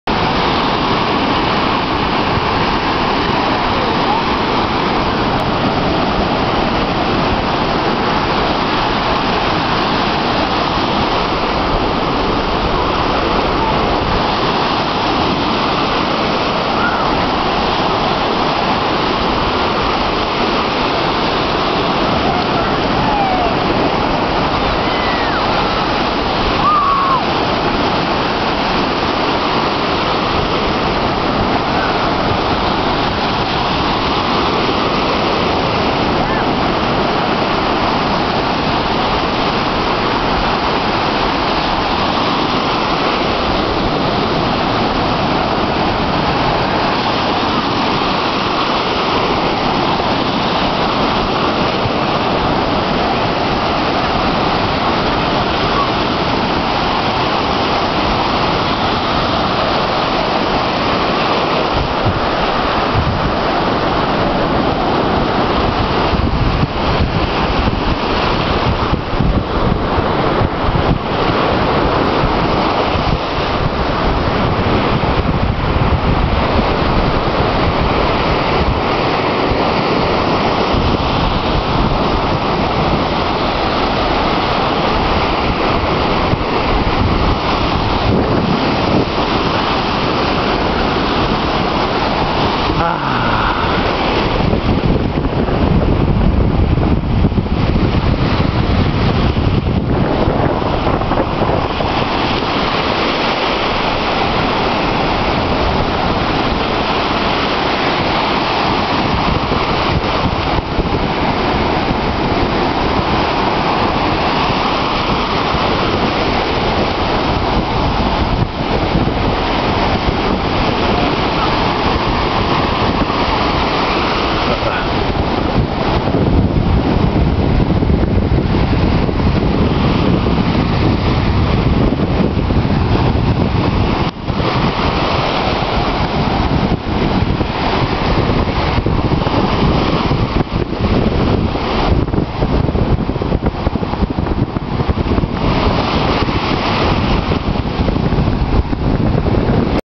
Boogie boarding at La Zebra in Tulum
Just a wonderful day in Tulum.  Boggie boarding and enjoying the scenery.  Love those waves